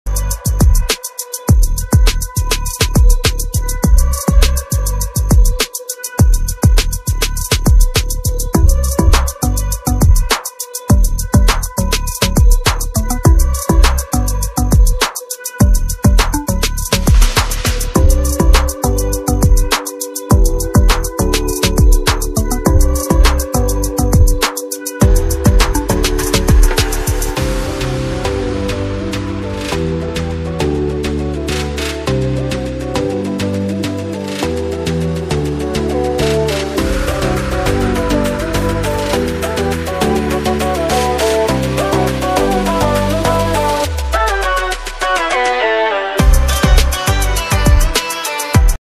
Diode Replacement for Jio phone Tips and Tricks for Mobile Repairing
Diode Replacement for Jio phone